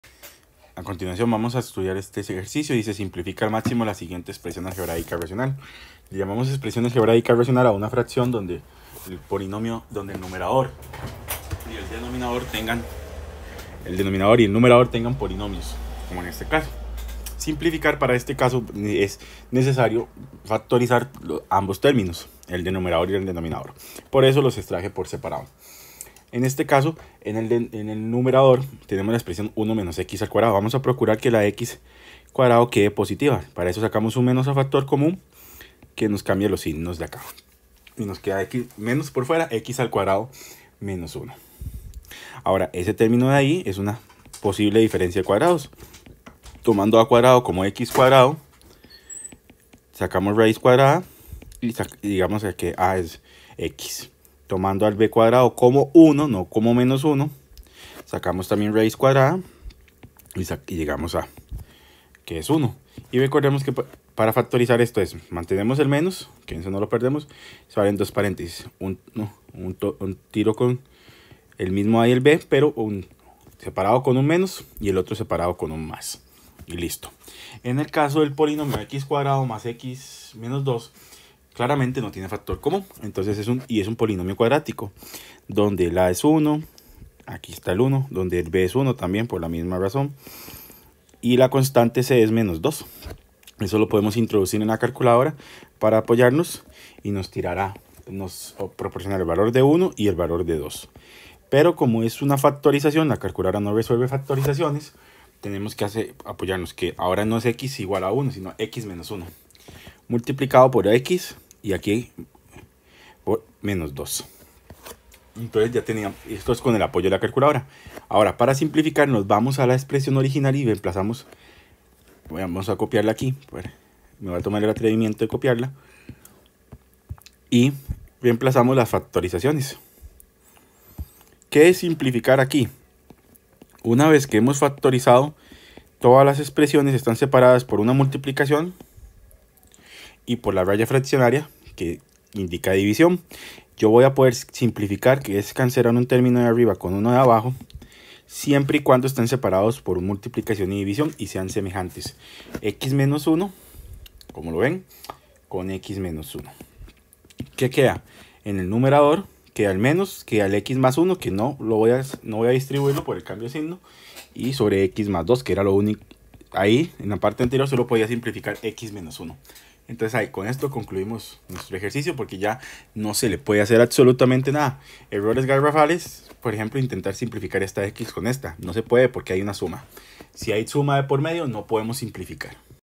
Ejemplo 1. Simplificación de expresiones algebraicas racionales
Ejercicio para simplificar una expresión algebraica racional